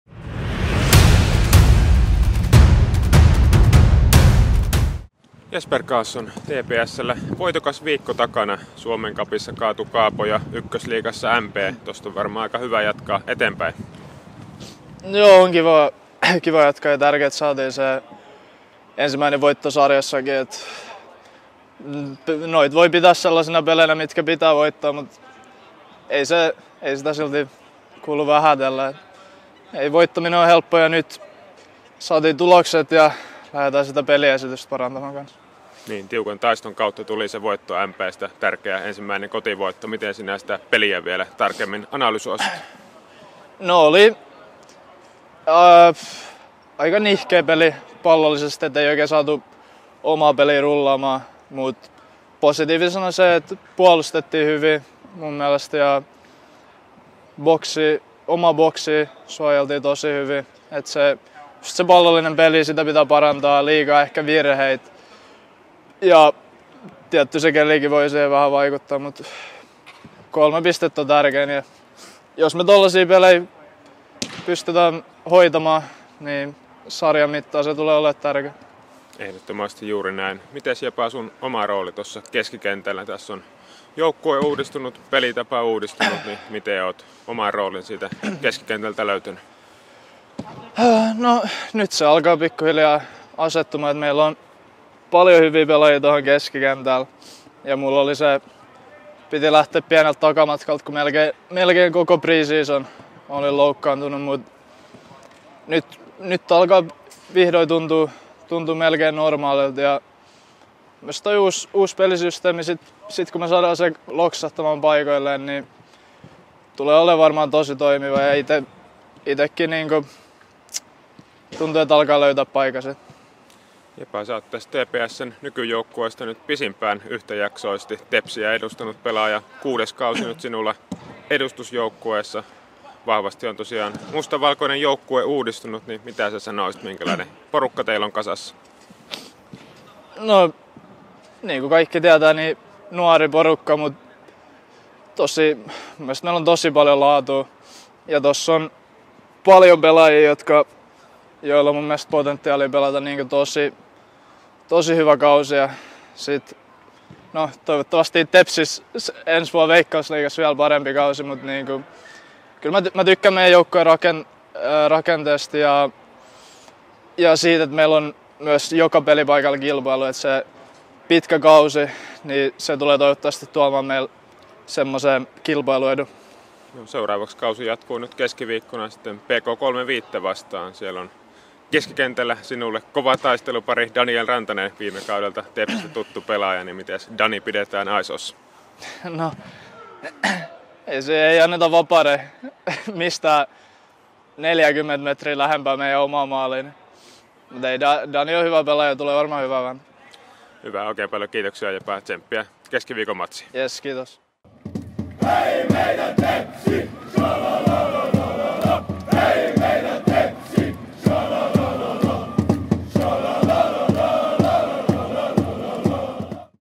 Ennakko: PK-35 – TPS | Ykkösliiga | 24.4.2024
Jalkapallon Ykkösliigan kolmas sarjakierros pelataan keskiviikkona. Tepsillä on edessään kauden ensimmäinen vieraspeli, kun Palloseura matkaa Helsinkiin kohtaamaan PK-35:n. Ottelu pelataan Pihlajamäessä Algeco Areenalla kello 18.30.

TPS TV:n ennakkohaastattelussa päästään tällä kertaa kuulemaan keskikenttäpelaaja Jesper Karlssonin mietteitä.